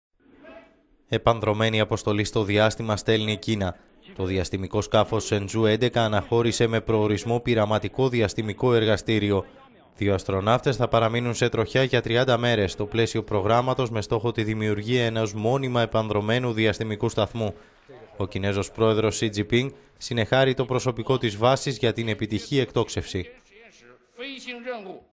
Κίνα: Σε τροχιά η μεγαλύτερη σε διάρκεια διαστημική αποστολή
Επανδρωμένη αποστολή στο διάστημα έστειλε η Κίνα.

Το διαστημικό σκάφος Σενζού 11 αναχώρησε με προορισμό πειραματικό διαστημικό εργαστήριο.

Δυο αστροναύτες θα παραμείνουν σε τροχιά για 30 ημέρες, στο πλαίσιο προγράμματος με στόχο την δημιουργία ενός μόνιμα επανδρωμένου διαστημικού σταθμού.

Η εκτόξευση μεταδόθηκε ζωντανά από την κρατική τηλεόραση.

Ο Κινέζος πρόεδρος Σι Τζινπίνγκ συνεχάρη το προσωπικό της βάσης Σιουκουάν, στην έρημο Γκόμπι για την επιτυχή εκτόξευση.…